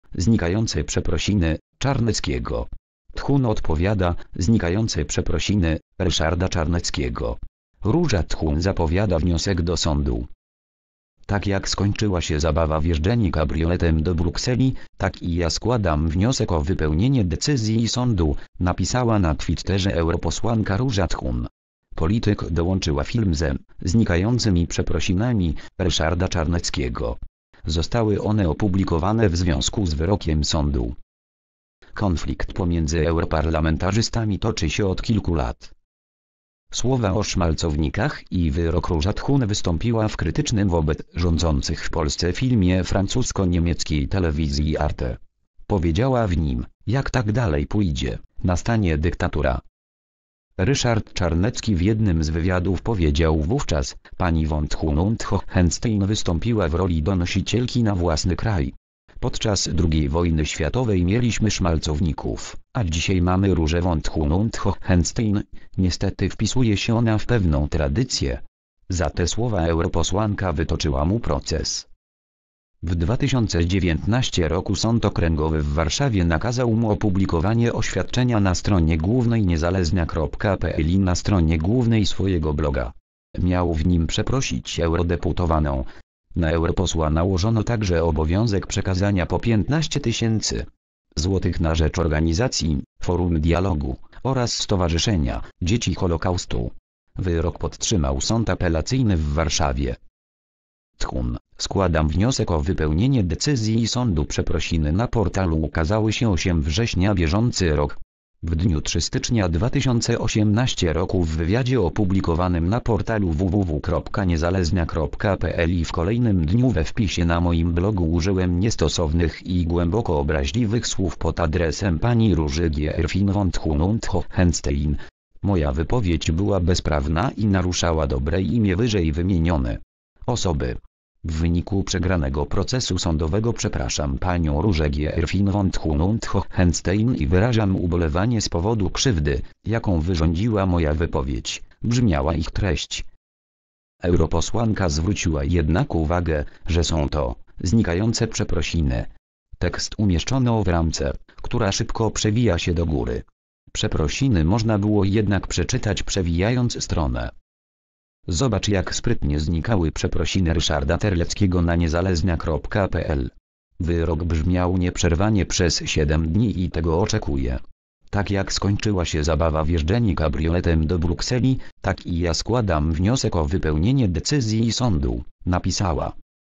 "Znikające przeprosiny" Czarneckiego. Thun odpowiada.
"Znikające przeprosiny" Ryszarda Czarneckiego. Róża Thun zapowiada wniosek do sądu.

   (fot. arch. PAP/Rafał Guz/PAP/Leszek Szymański)